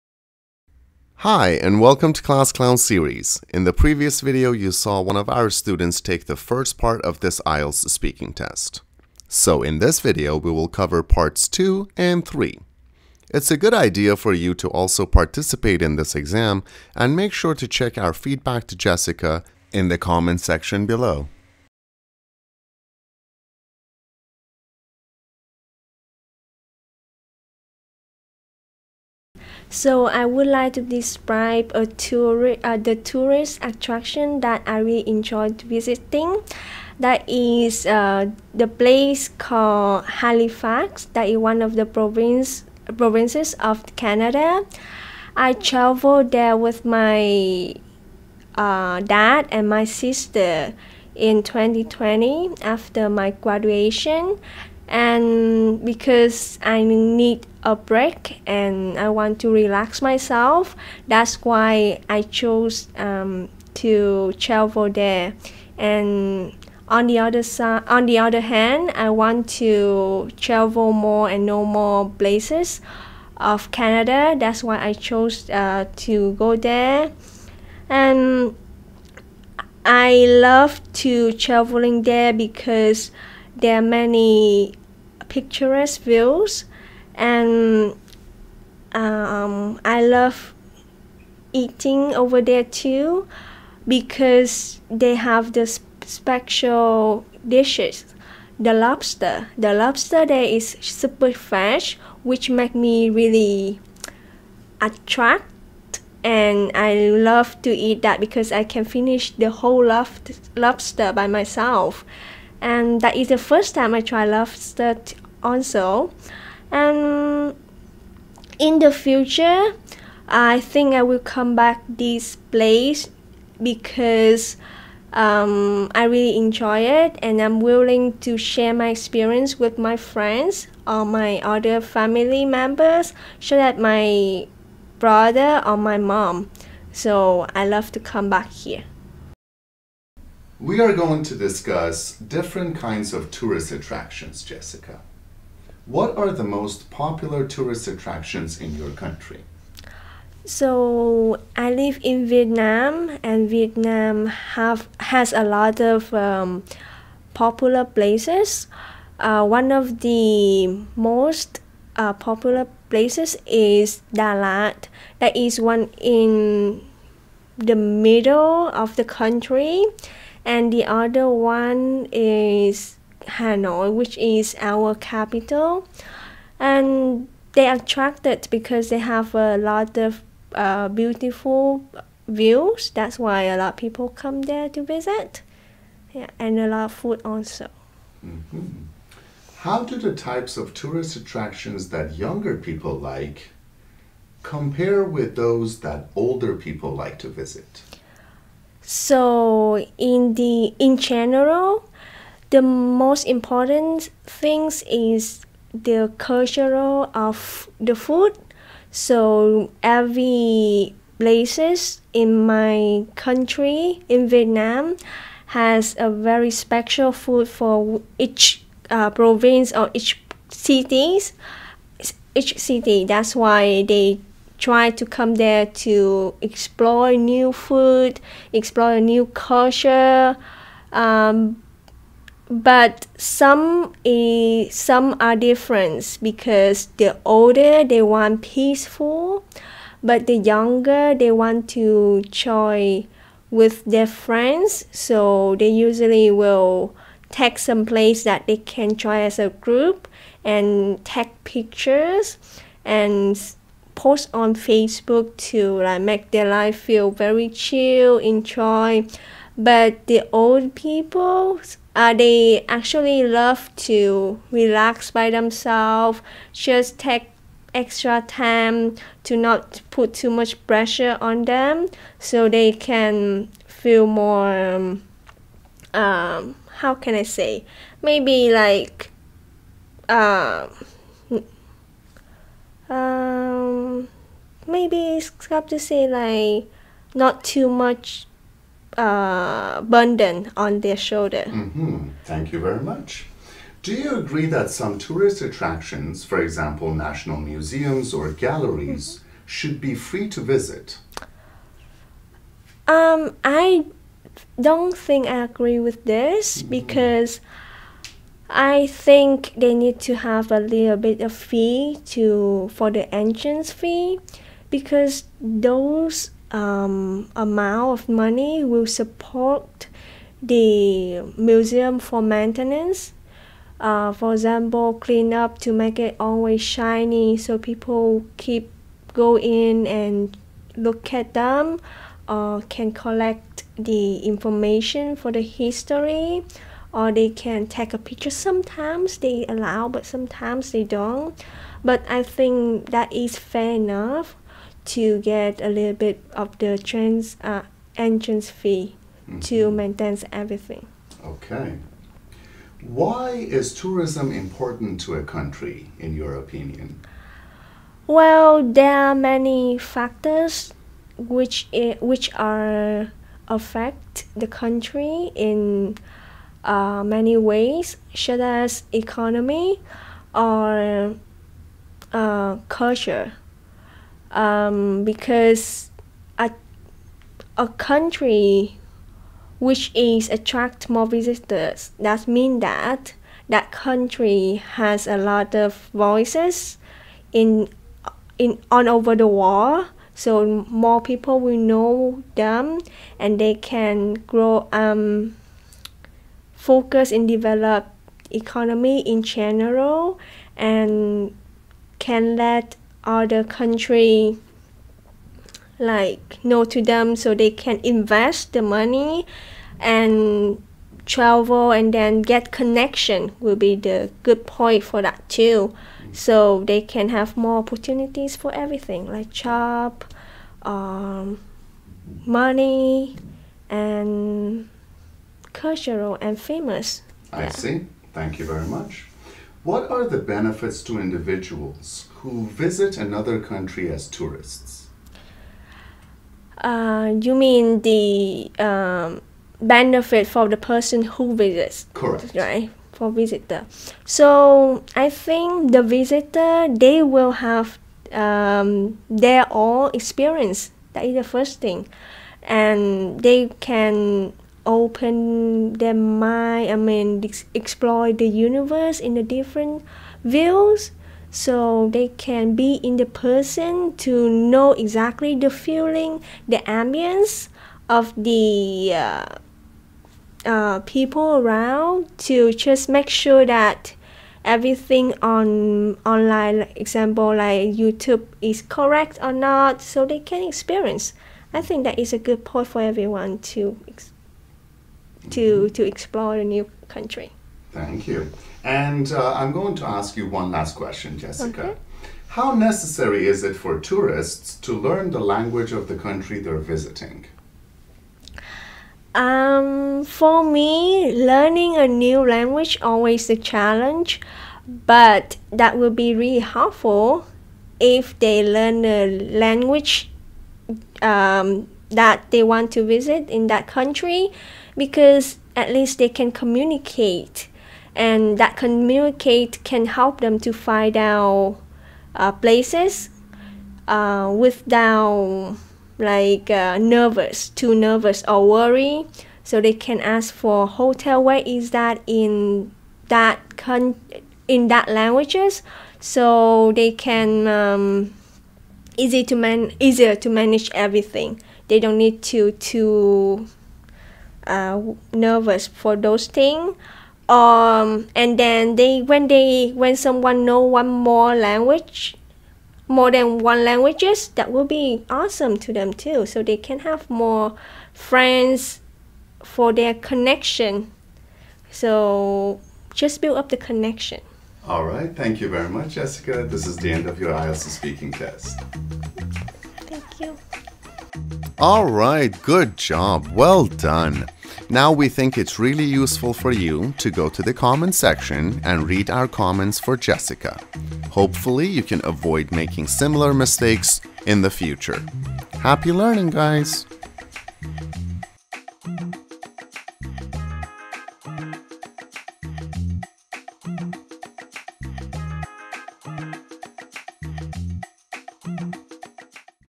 IELTS speaking parts 2 & 3 on the topic of 'tourism' - Practice with a Vietnamese student!
Some of Jessica's mistakes in part 2:

Jessica: … in 2020... because I need a break and I want to relax myself...
CCS: …because I needed a break and wanted to relax...

J: …that's why I choose to travel...
CCS: … that's why I chose to travel...

J: …I want to travel more and know more places...
CCS: I wanted to travel more and get to know more places...

J: I love to traveling there...
CCS: I loved traveling there...

J: …the lobster... which make me really attract...
CCS: …which is very attractive to me...

J: …I would come back this place...
CCS: …I would come back to this place...

Some of Jessica's mistakes in part 3:

J: …they're attracted because...
CCS:...those places are very attractive...

J: …the cultural of the food...
CCS: …the food culture...

J: every places...
CCS: every place...

J: …some are difference...
CCS:...some are different...

J: …they want peaceful
CCS:...they want peace and quiet

J: …very chill and enjoyed...
CCS:...very chill and enjoyable...

J: …to maintenance everything ...
CCS:...to maintain everything... or to pay for maintenance fees...

J: …there are many factors which are affect the country...
CCS:...there are many factors that can affect a country...

J: …a country which is attract more visitors...
CCS:...a country which attracts more visitors...

J: …that's means that...
CCS: …that means that...

J: …like job, money, and cultural and famous...
CCS: …like job, money, culture and fame...

J: …learning a new language always a challenge...
CCS: …learning a new language is always a challenge...

J: …that communicate can help them...
CCS: …that communication can help them...

J: ...find out place without nervous...
CCS: ...find out about different places without getting nervous...

J: …for those thing...
CCS: …for those things...

J: …when someone know more than one languages...
CCS: …when someone knows more than one language...

Did you catch any more mistakes? How about pronunciation errors?